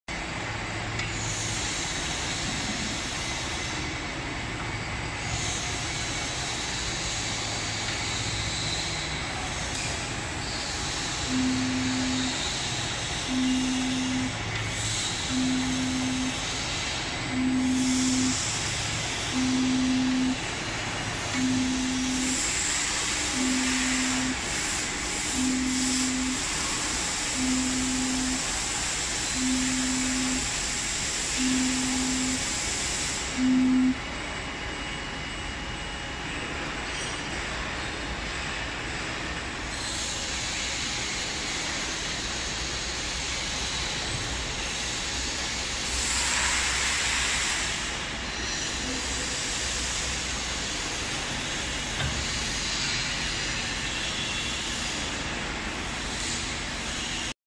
Heavy Duty Reel Wrapping Machine
Reel wrapper is designed for packing paper rolls, woven fabrics etc. This machine provide a cocoon type packing and protect the product from damage, moisture and dust.

We can tailor made according your paper rolls size and weight.